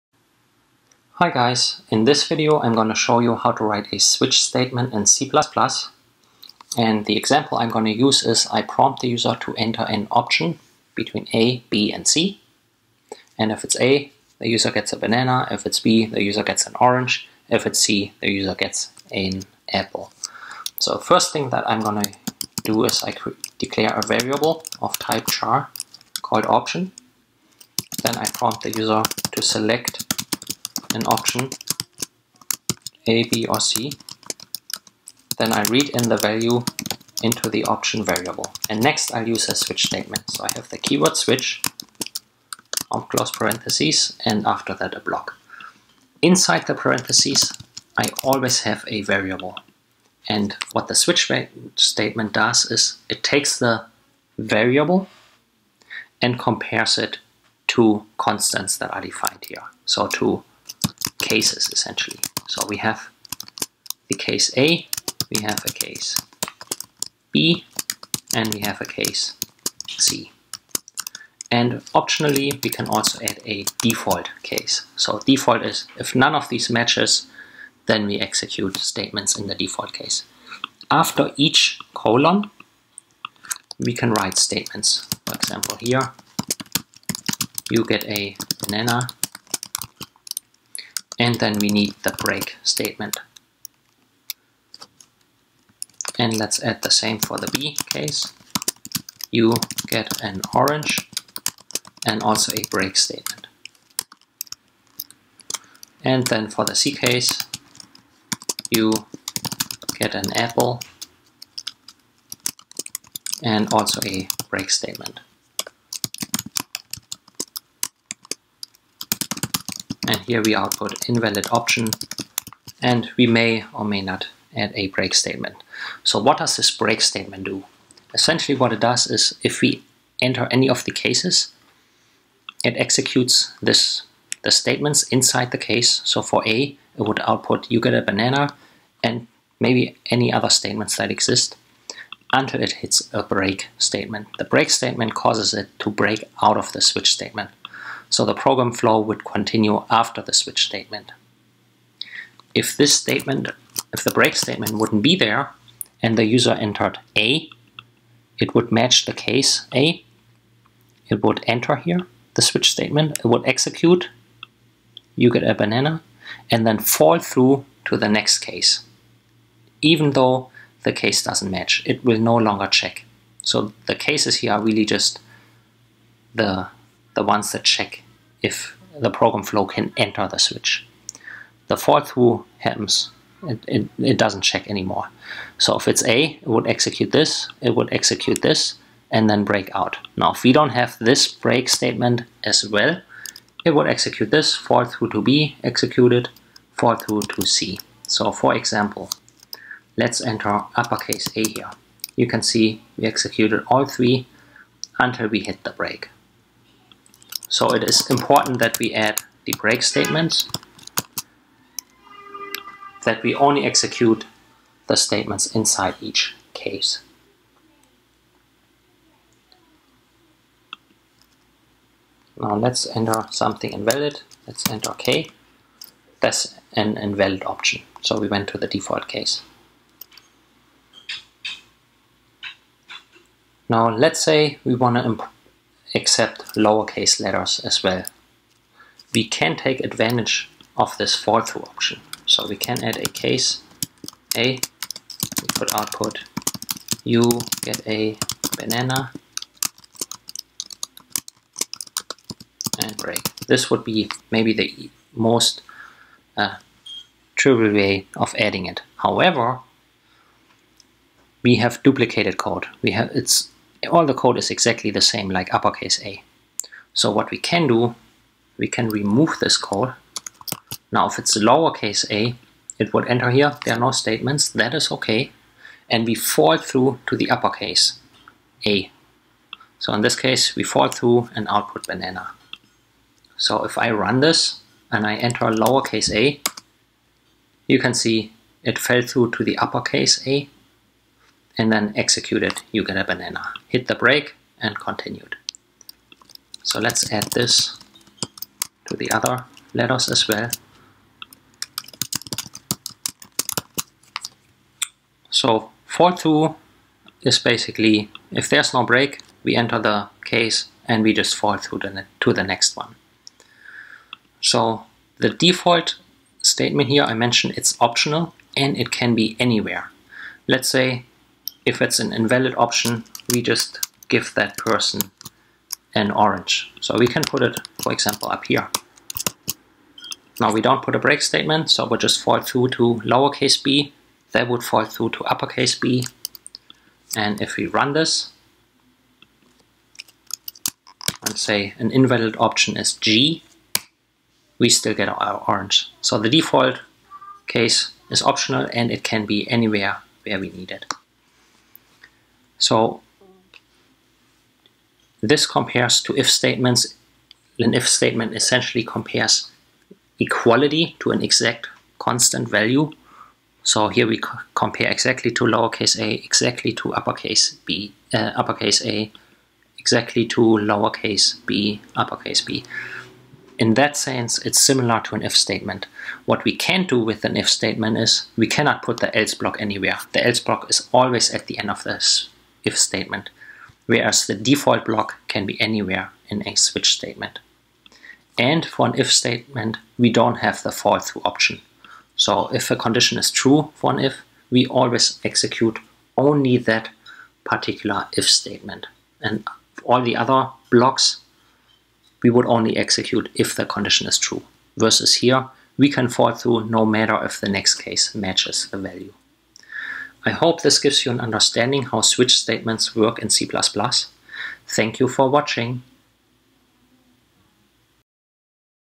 C++ Tutorial: Switch statement
This video is a tutorial on using switch statements in C++